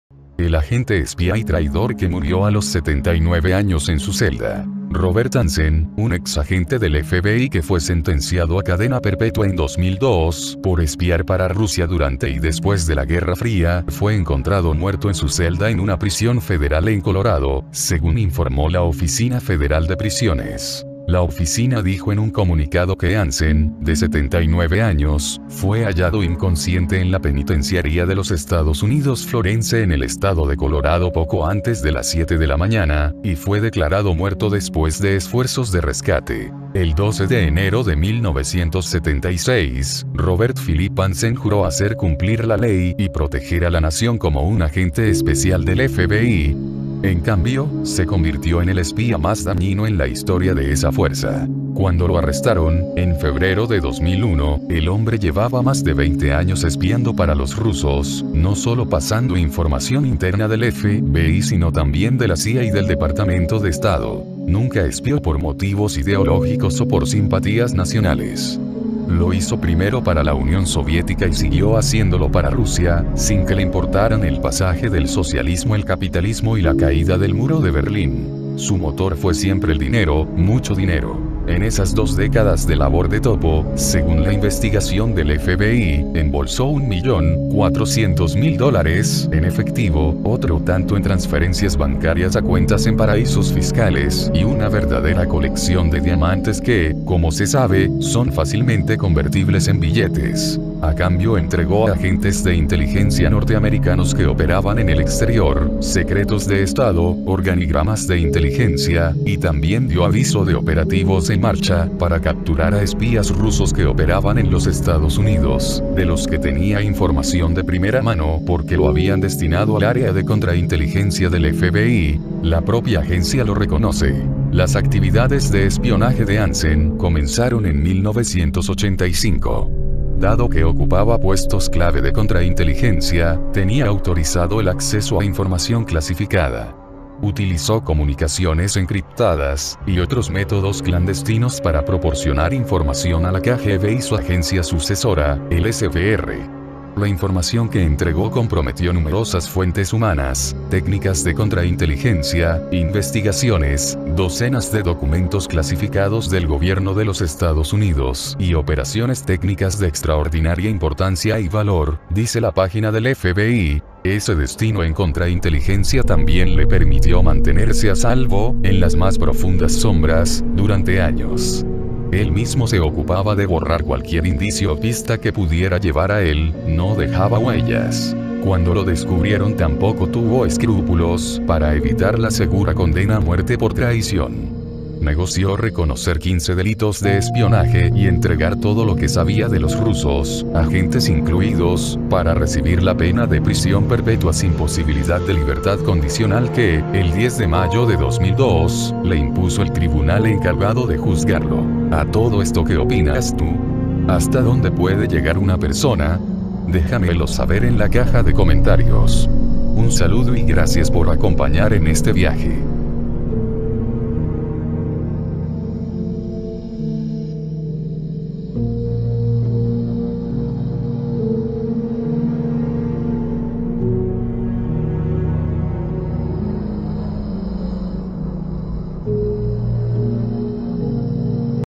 Relatos de Terror - El agente espía que traicionó a su país por dinero
Murió en prisión Robert Hanssen, el agente del FBI que se hizo millonario revelando secretos a Rusia durante 25 años
El 10 de mayo de 2002, la justicia estadounidense lo condenó a a prisión perpetua sin posibilidad de libertad condicional por “traición”